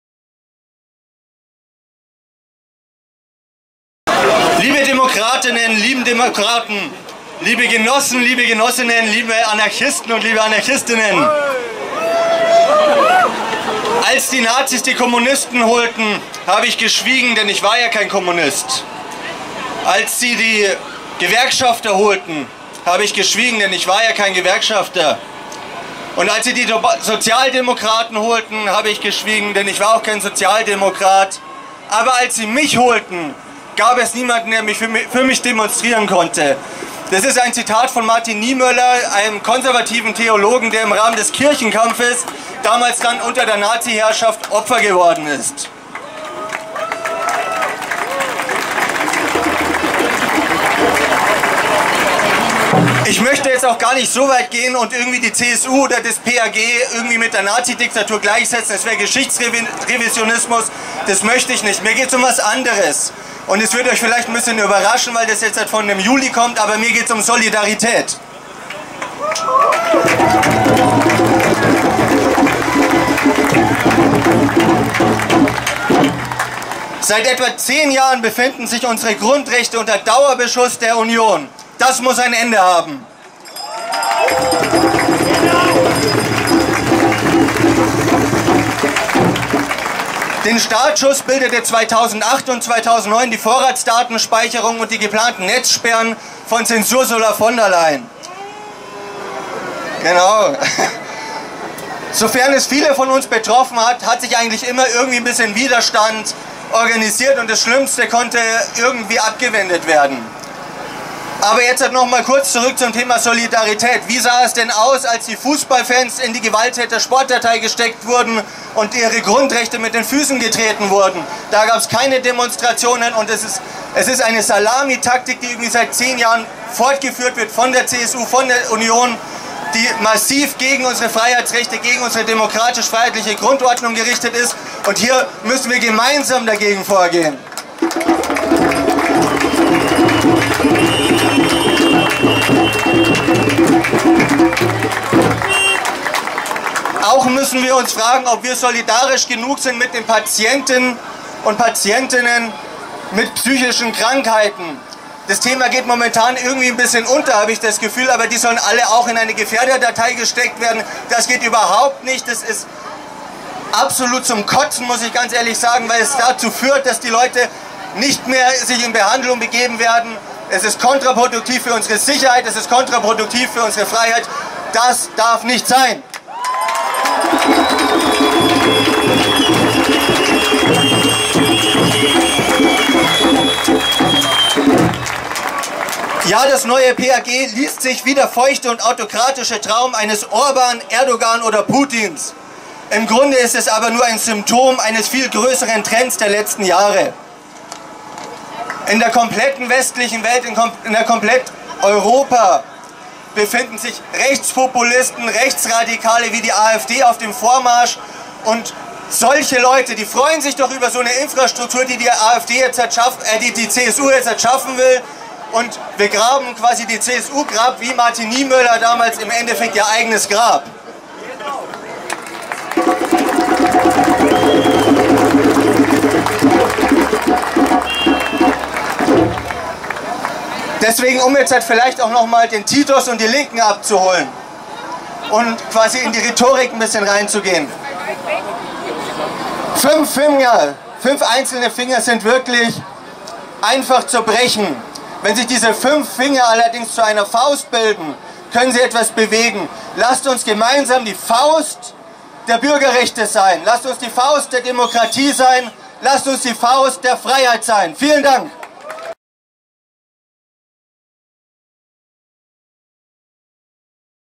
Der bayerische Kampfgeist ist geweckt - Rede von Simon Landenberger, JuLis / FDP
Der bayerische Kampfgeist ist geweckt -
Breites Bündnis mobilisiert in Nürnberg 6000 Menschen gegen das Polizeiaufgabengesetz

Nürnberg. Mit soviel Zulauf hatten selbst die VeranstalterInnen nicht gerechnet: In der Nürnberger Südstadt protestierten am Freitag, 20. April 2018, bis zu 6000 Menschen gegen das geplante bayerische Polizeiaufgabengesetz (PAG) unter dem Motto: „Nein zum neuen Polizeiaufgabengesetz! Grundrechte schützen!“. Zu der Demonstration, die am Aufseßplatz begann, hatte ein Bündnis von mehr als 40 Organisationen und Parteien aufgerufen – unter anderem Grüne, Linke, SPD und FDP, aber auch weitere Oppositionsparteien, Bürgerrechtler und Journalistenvereinigungen.